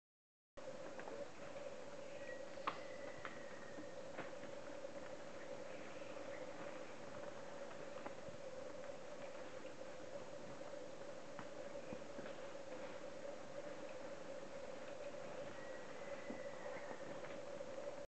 Squirrel Monkey. Grijsgroen Doodshoofdaapje. Reserva Ecologica Taricaya, Peru, 25 May 2009.MOV
Squirrel Monkey (in Dutch: Grijsgroen Doodshoofdaapje). Reserva Ecologica Taricaya, Peru, 25 May 2009.